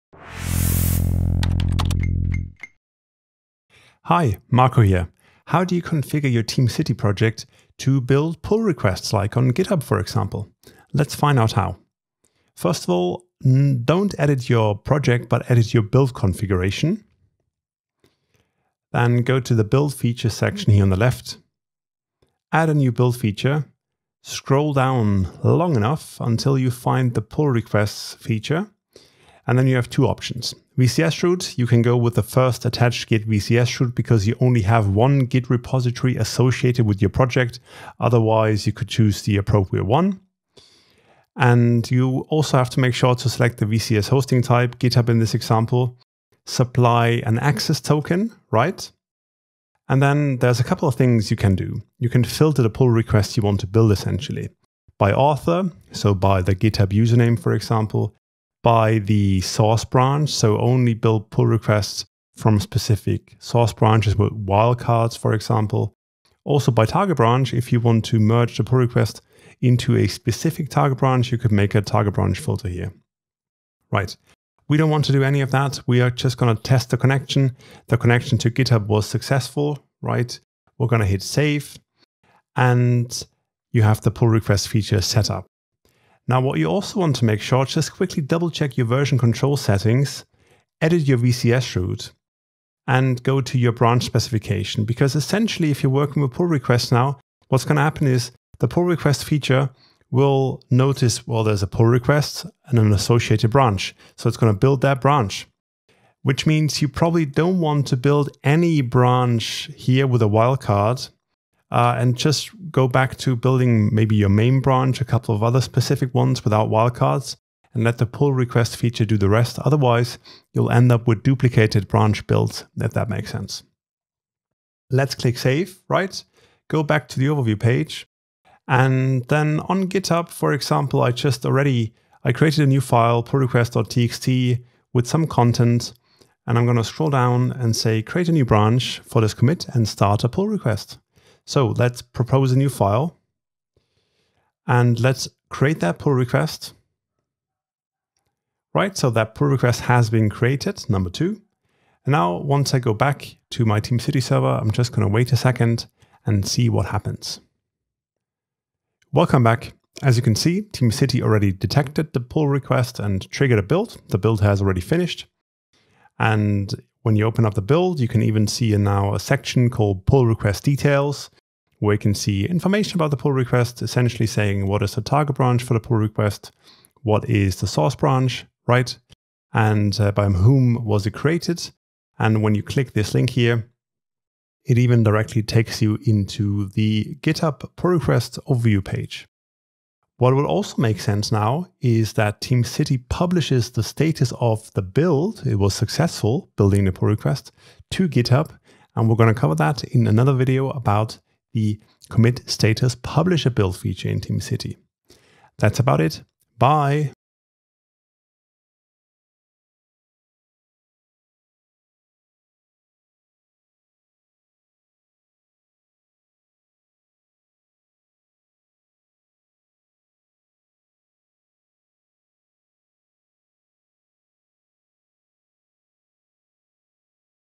TeamCity tutorial - How to work with pull requests
How do you configure your CI/CD jobs to build pull requests? The Pull Requests build feature in TeamCity makes it possible to automatically detect and build pull requests from GitHub, GitLab, Bitbucket, and Azure DevOps.

In this tutorial, we go over configuring this for GitHub, and show the feature in action by creating a pull request and having it built by TeamCity.

0:00 Adding the Pull Request build feature
0:43 GitHub pull request filtering
1:26 VCS root branch specification
2:04 Creating and building a pull request